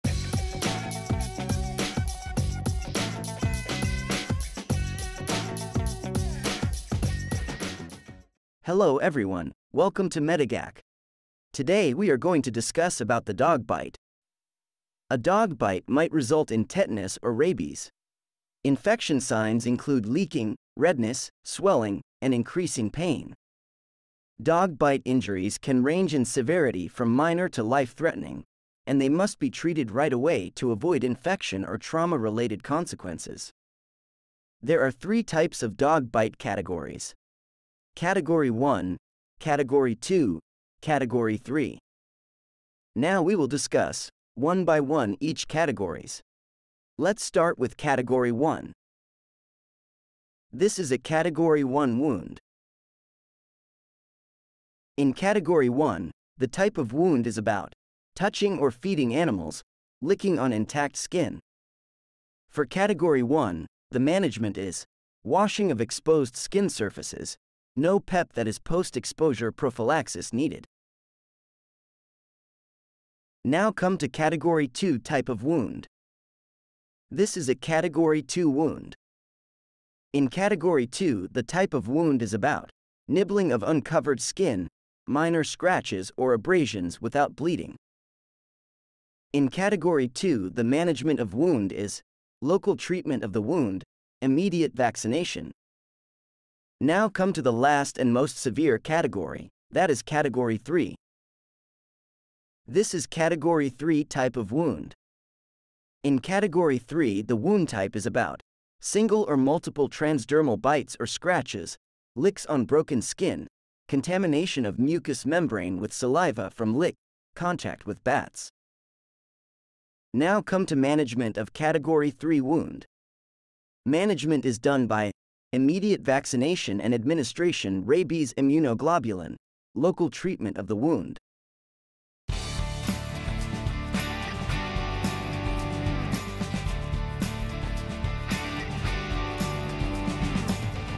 Dog bite - Category 1,2,3 || Wound types and Management/Treatment
We have discussed all three categories-I,II,III of dog bites, their wound types and management.